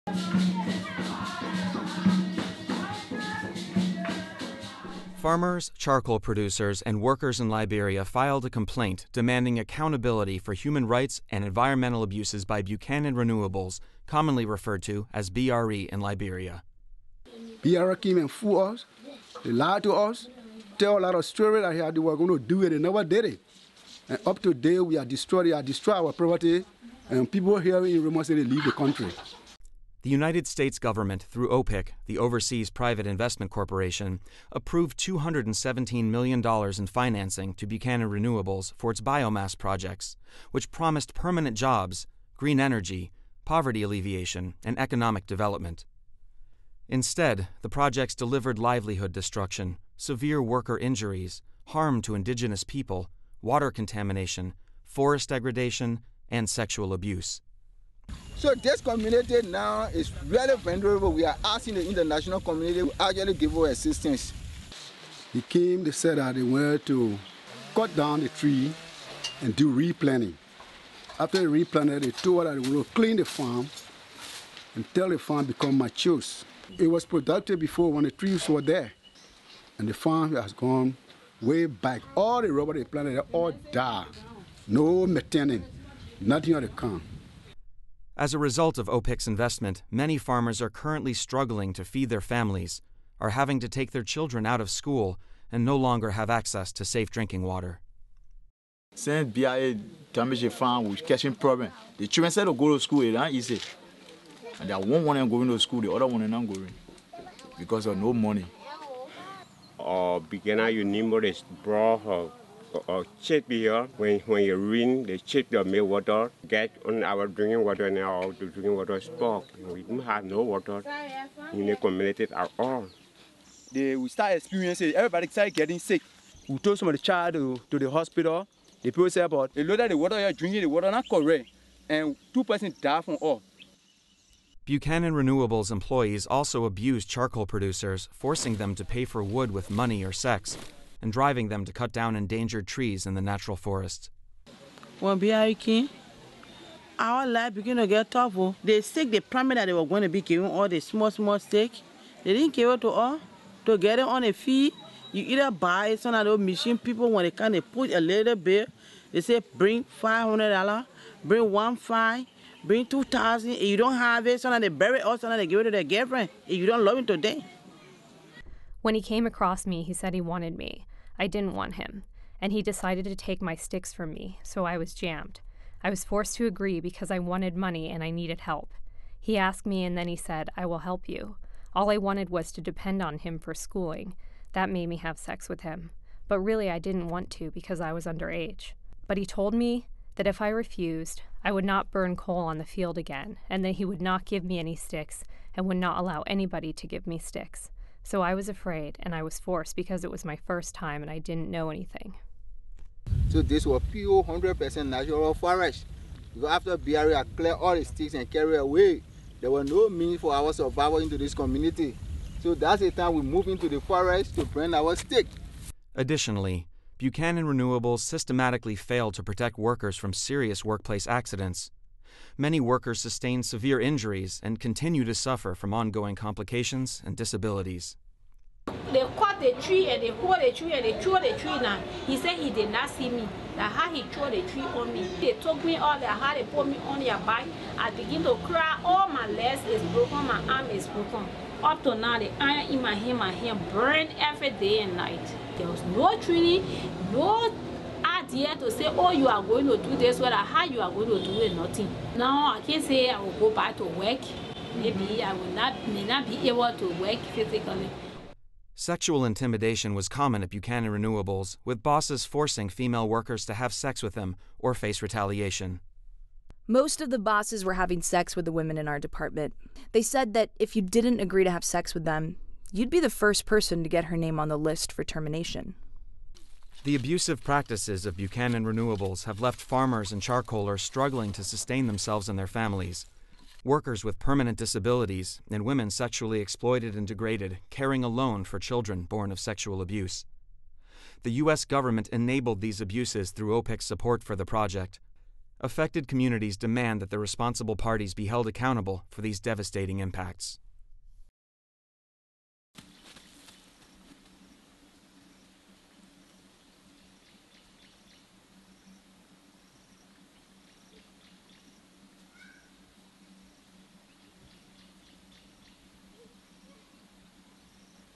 OPIC's Fueling of Human Rights Disasters in Liberia
On January 22, 2014, hundreds of Liberians filed a complaint with a U.S. Government agency demanding accountability for its funding of a biomass company that caused serious human rights, labor and environmental abuses, including sexual abuse by company employees of local women. The U.S. Government agency -- the Overseas Private Investment Corporation (OPIC) --approved over $200 million in financing for the company, Buchanan Renewables.  Tell OPIC's President and CEO that her agency must take responsibility for its role in funding this human rights disaster by signing our petition.